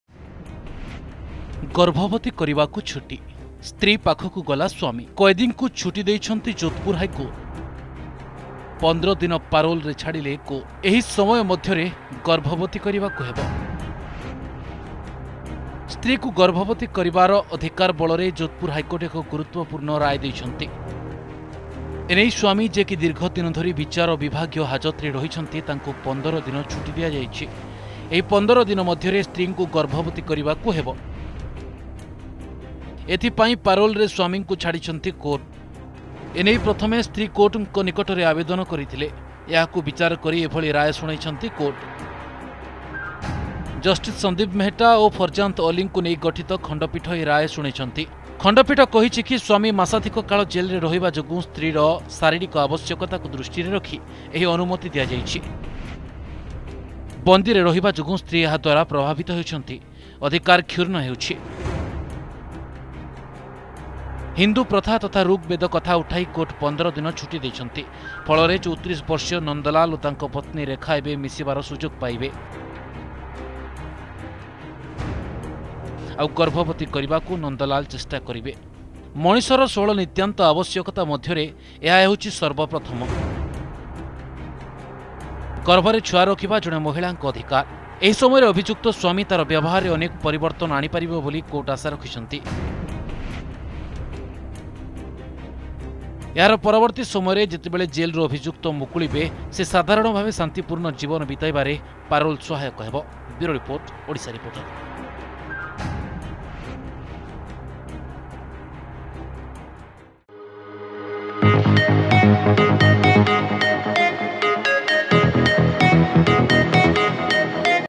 ପତ୍ନୀଙ୍କୁ ଗର୍ଭବତୀ କରିବାକୁ କୋର୍ଟ ଦେଲେ ଛୁଟି | Odisha Reporter
ସ୍ତ୍ରୀକୁ ଗର୍ଭବତୀ କରିବାର ଅଧିକାର ବଳରେ ଯୋଧପୁର ହାଇକୋର୍ଟ ଏକ ଗୁରୁତ୍ୱପୂର୍ଣ୍ଣ ରାୟ ଦେଇଛନ୍ତି। ଏନେଇ ସ୍ୱାମୀ ଯେ କି ଦୀର୍ଘଦିନ ଧରି ବିଚାର ବିଭାଗୀୟ ହାଜତରେ ରହିଛନ୍ତି ତାଙ୍କୁ 15 ଦିନ ଛୁଟି ଦିଆଯାଇଛି। ଏହି 15 ଦିନ ମଧ୍ୟରେ ସ୍ତ୍ରୀଙ୍କୁ ଗର୍ଭବତୀ କରିବାକୁ ହେବ।